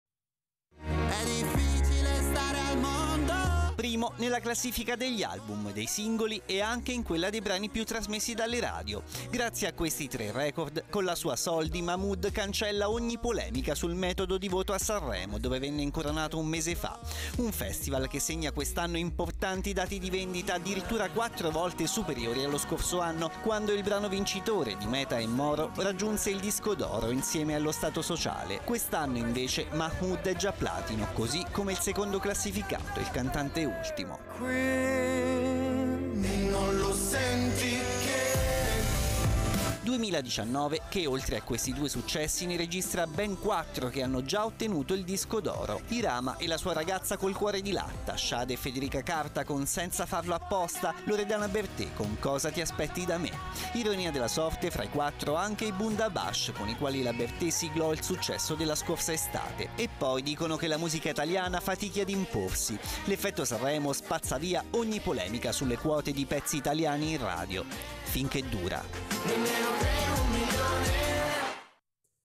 Sanremo boom di vendite delle canzoni
E’ boom di vendite per le canzoni dell'ultima edizione di Sanremo. Oltre 225 mila le copie dei singoli vendute nell'ultimo mese, quattro volte in più rispetto allo stesso periodo del 2018. Il servizio di Silvio Vitelli